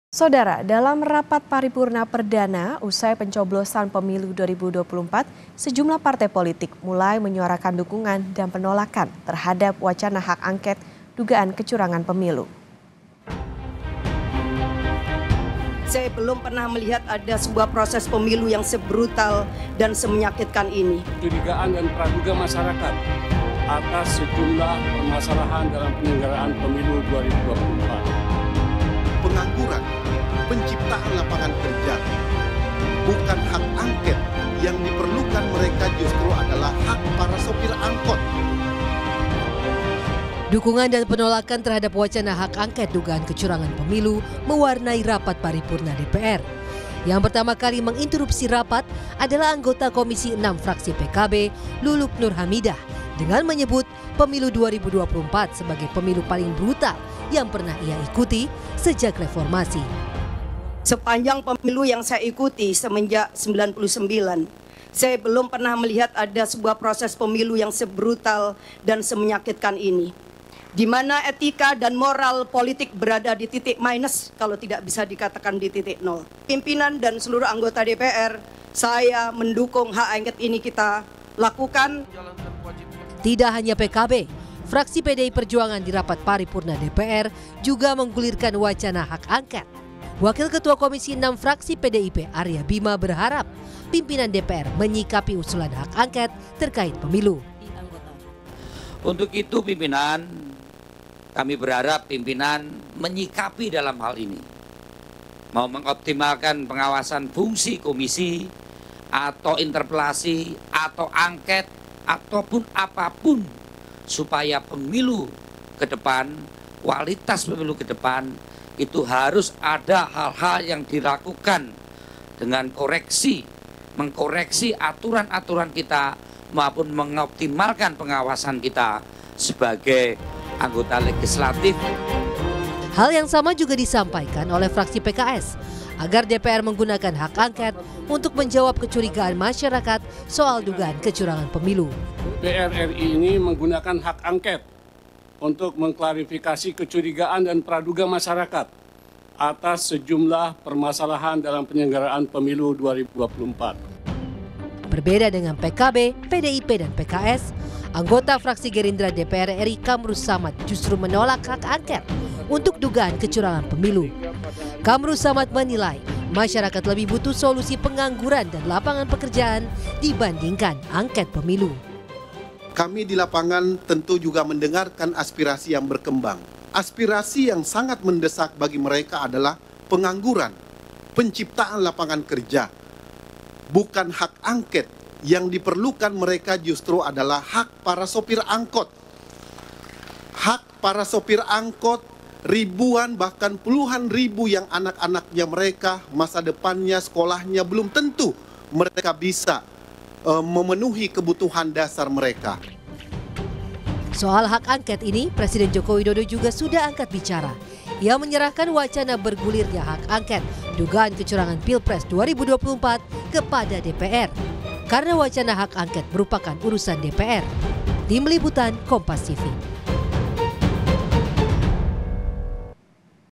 Momen Anggota DPR Debat soal Pengajuan Hak Angket Dugaan Kecurangan Pemilu 2024 di Rapat Paripurna
KOMPAS.TV - Dalam rapat paripurna perdana usai pencoblosan Pemilu 2024, sejumlah partai politik mulai menyuarakan dukungan dan penolakan terhadap wacana hak angket dugaan kecurangan pemilu.

Yang pertama kali mengitrupsi rapat adalah anggota Komisi VI fraksi PKB Luluk Nur Hamidah dengan menyebut Pemilu 2024 sebagai pemilu paling brutal yang pernah ia ikuti sejak reformasi.

Tidak hanya PKB fraksi PDIP di rapat paripurna DPR juga menggulirkan wacana hak angket. Wakil Ketua komisi VI fraksi PDIP Aria Bima berharap pimpinan DPR menyikapi usulan hak angket terkait pemilu.

Berbeda dengan PKB, PDIP, dan PKS, aanggota fraksi Gerindra DPR RI Kamrussamad justru menolak hak angket untuk dugaan kecurangan pemilu.

Kamrussamad menilai, masyarakat lebih butuh solusi pengangguran dan lapangan pekerjaan dibandingkan angket pemilu.

Kompas TV
Independen | Terpercaya